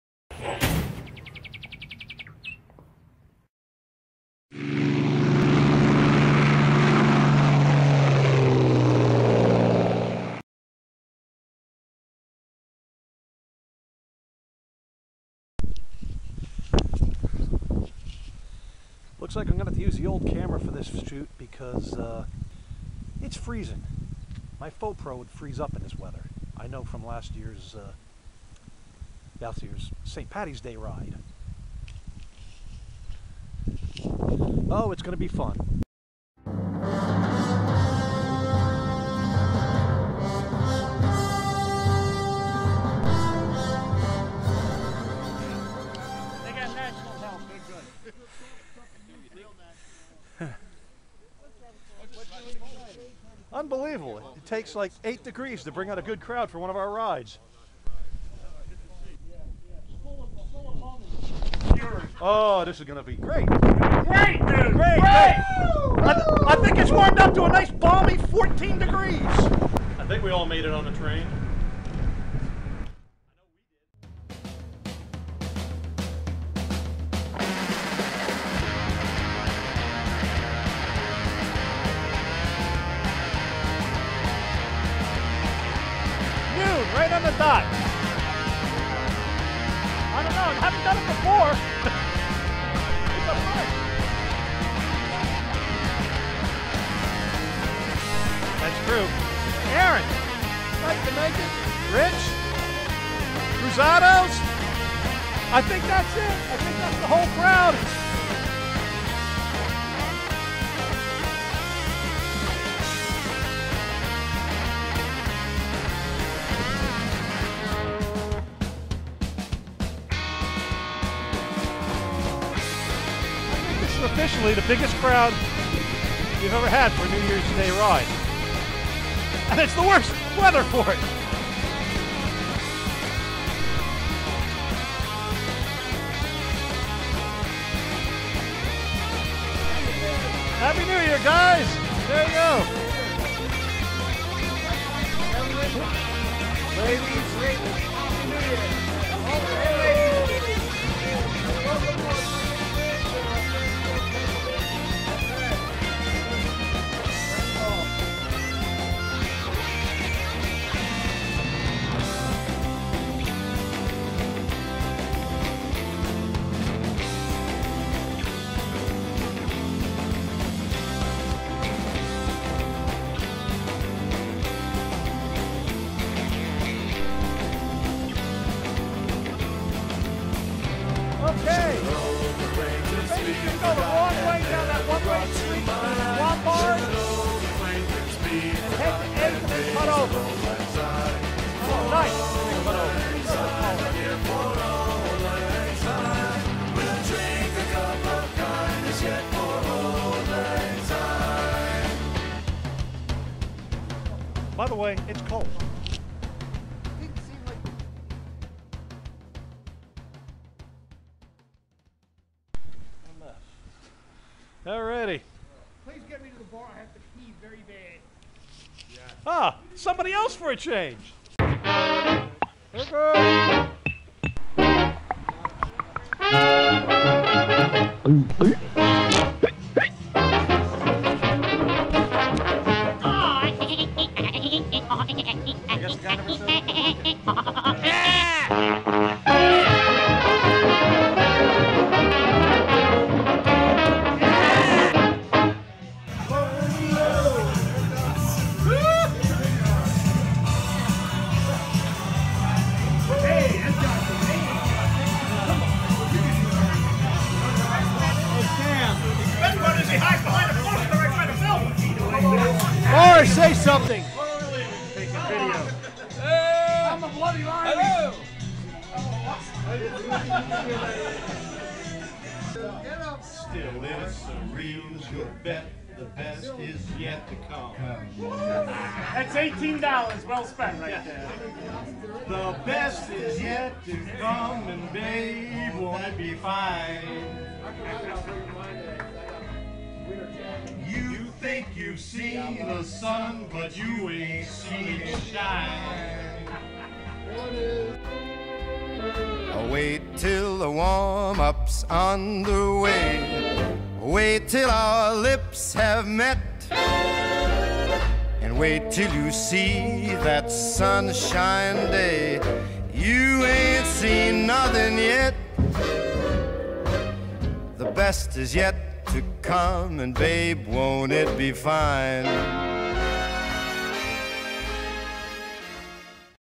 New Years Day Pub Ride 2018
It's the 7th or 8th annual New Year's Day Pub Ride in Philadelphia and South Jersey! Not even sub freezing temperatures stopped us!